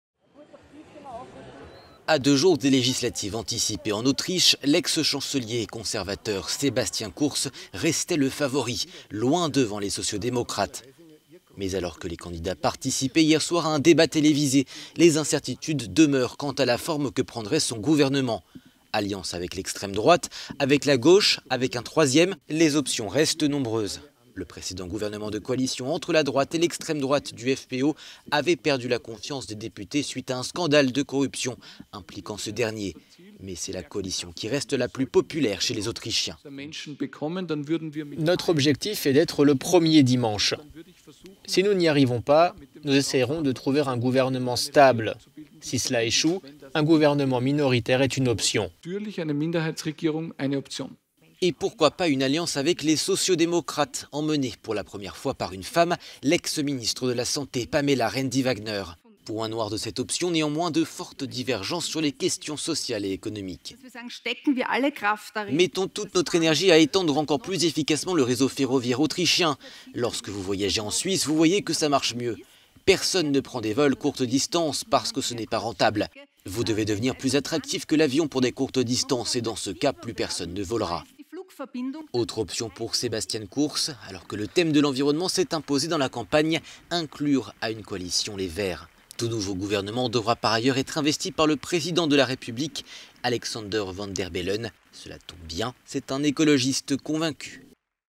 J-2 avant les législatives en Autriche : quelles options pour former un gouvernement ?
L'ex-chancelier Sebastian Kurz, qui reste le favori, loin devant les sociaux démocrates, a déclaré qu'il n'excluait pas un gouvernement "minoritaire".…